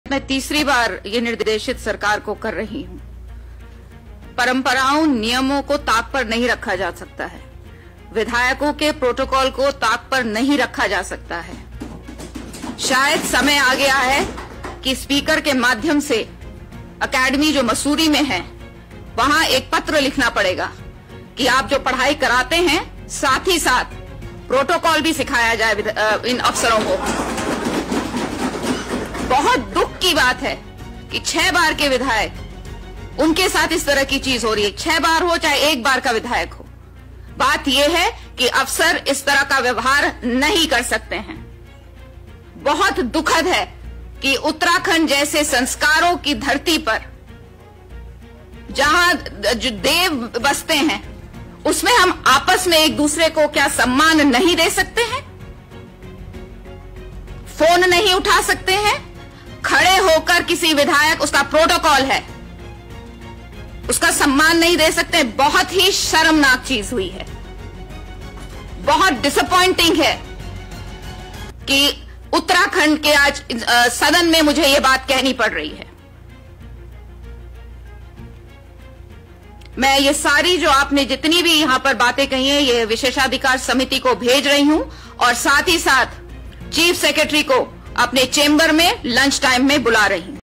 विधानसभा अध्यक्ष ने सरकार को जमकर सुनाया
विधायकों पर अफ़सरशाही हावी होने को लेकर विधानसभा अध्यक्ष ने सरकार को जमकर सुनाया